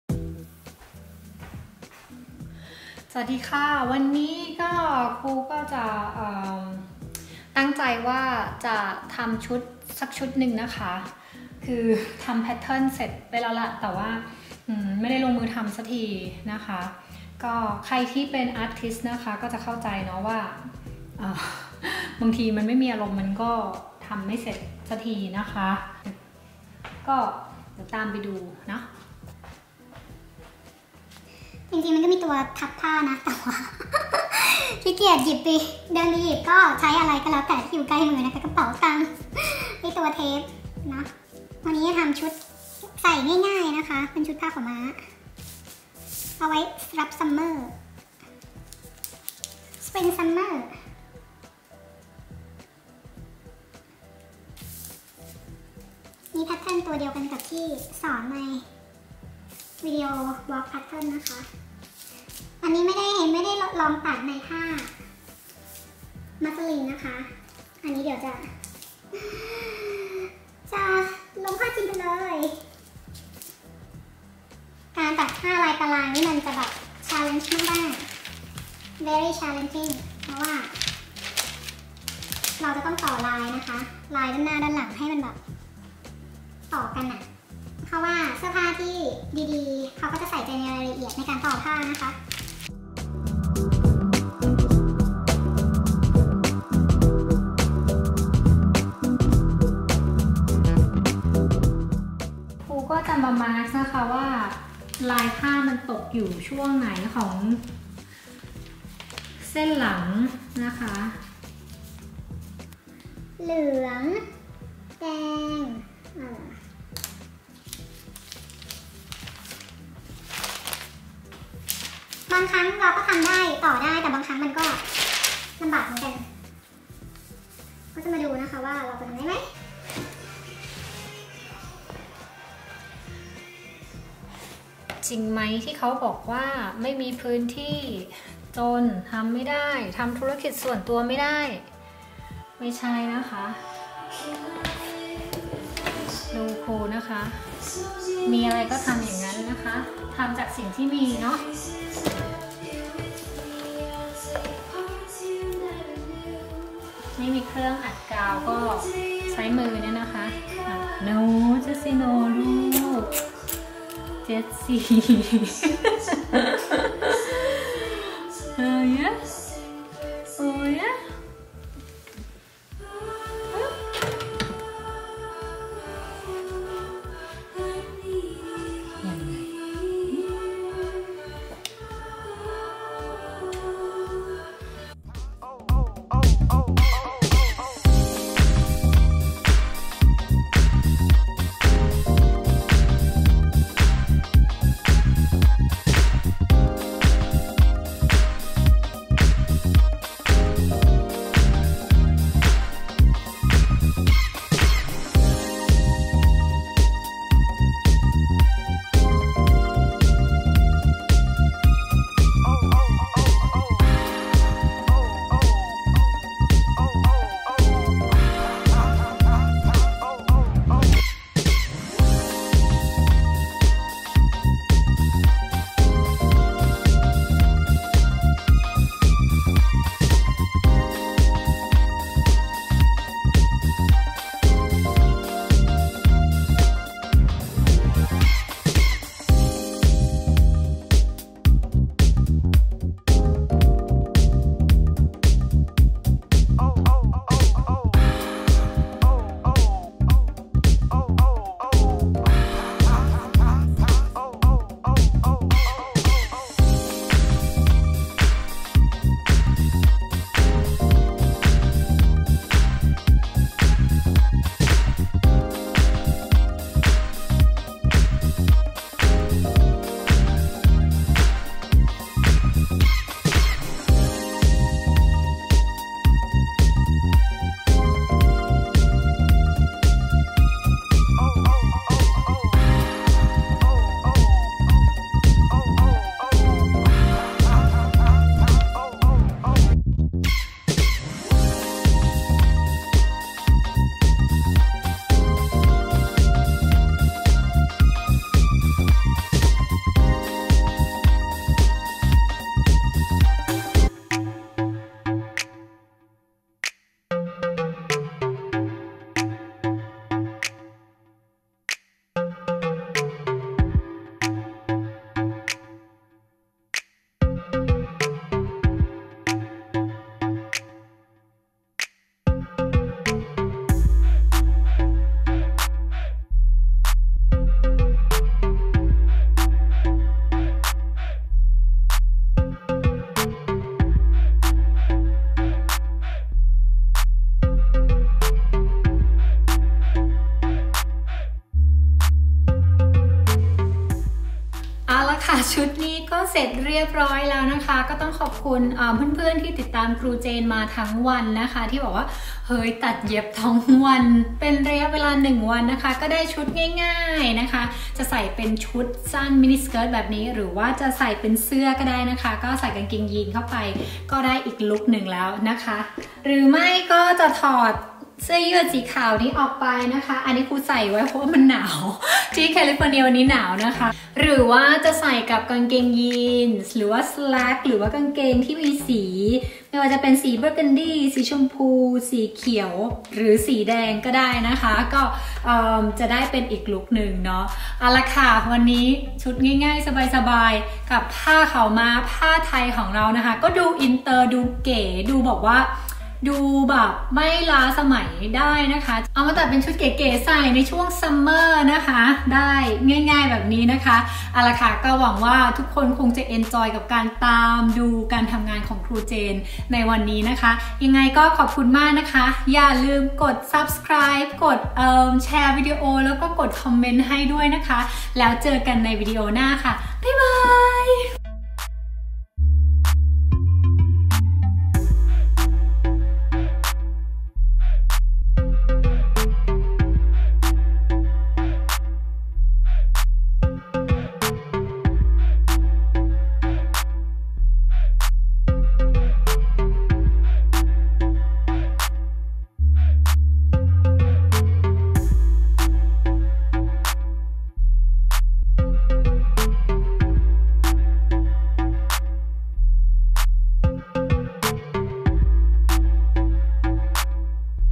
1 ชุด 1 วัน มาดูกันเสร็จไหม ชุดผ้าขาวม้าไทย
ตามมาดูการทำงานของครูเจนในวันตัดเย็บนะคะ ชุดผ้าขาวม้าออกแบบง่ายๆให้ใส่ได้ไสตล์โมเดิร์น แบบใส่ได้ทุกวันทุกโอกาส สีอาจจะเหมาะกับช่วง spring summer นะคะ

หากวิดีโอนี้สร้างความบันเทิงและเป็นประโยชน์ต่อเพื่อนๆ รบกวน Thumb Up คลิกไลค์ให้ป้าเจนด้วยนะคะ และจะขอบพระคุณอย่างสูงหากคลิกแชร์วิดีโอต่อๆไปด้วยอะคะ _/\_